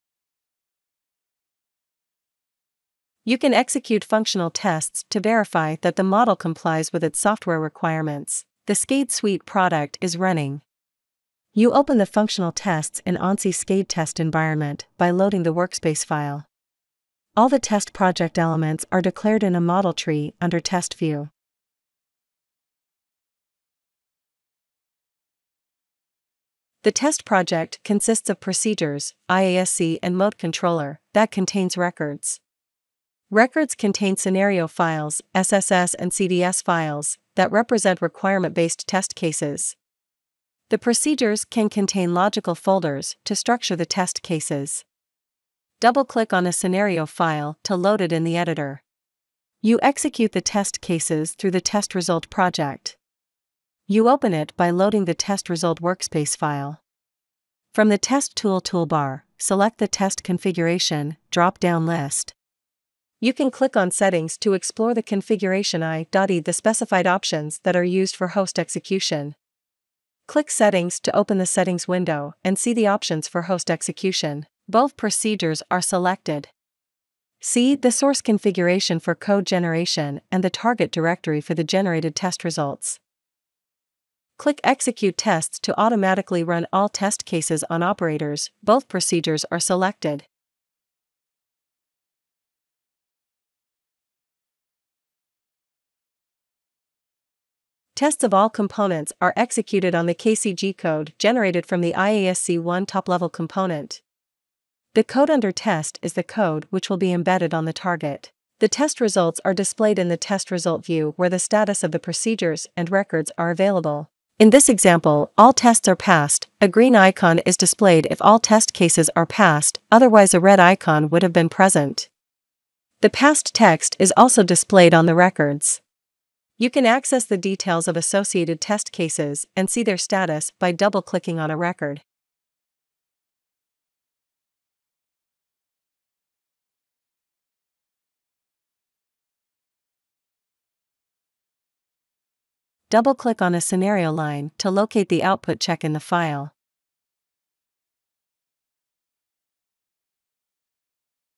Introduction to Ansys SCADE Test Environment Tutorial 5
Ansys SCADE Suite - Cabin Pressure Control System project tutorial video 5 for SCADE Suite Trial.